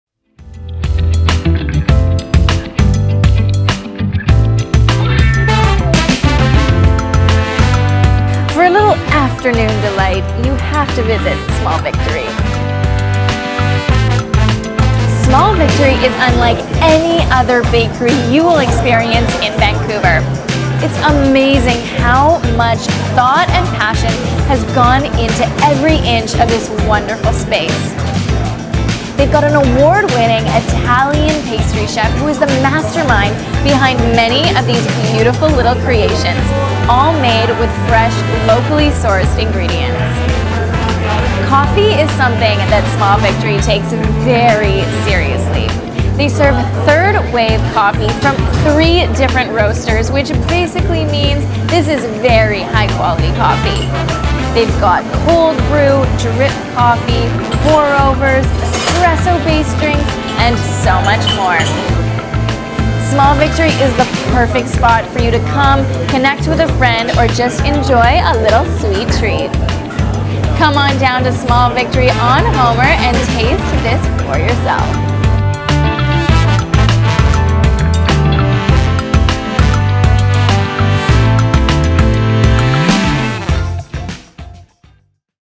Small Victory Bakery Cafe Coffee Shop Vancouver BC serving Dessert and Salad
Small Victory
1088 Homer St, Vancouver BC Canada V6B 2W9

Small Victory savouring fresh baked bread and fantastic coffee is a definite victory over the rest of your day.